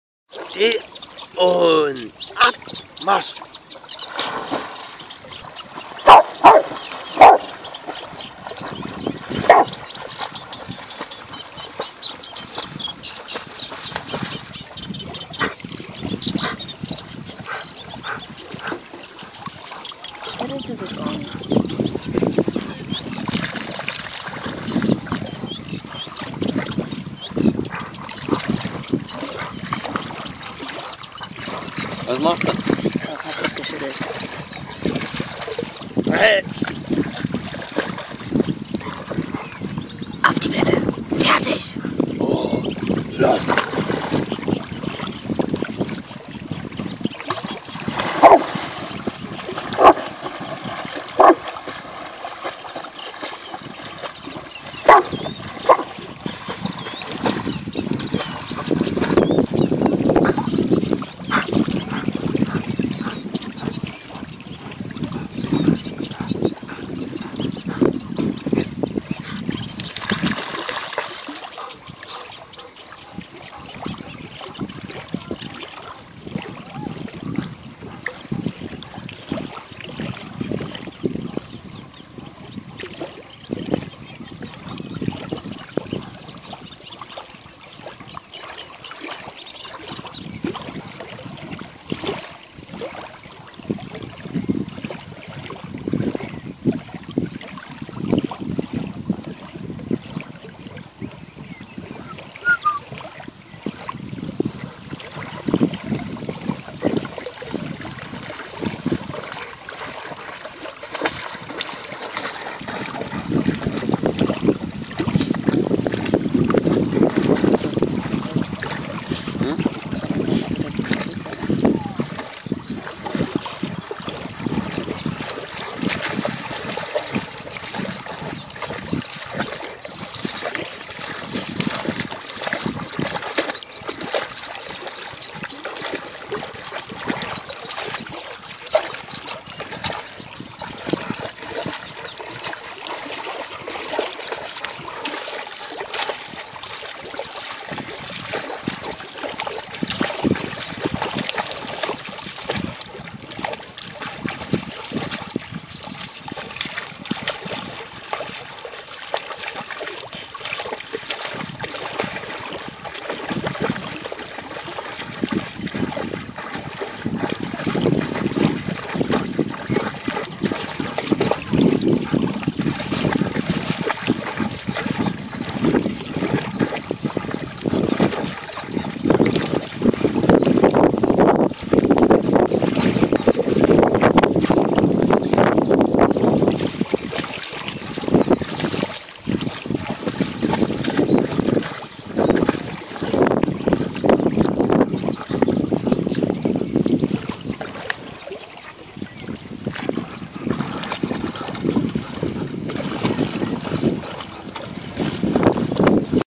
Rottweiler beim planschen
Tapsi in ihrem Revier baden an der kiesi !!! Unsre Wasserratte !!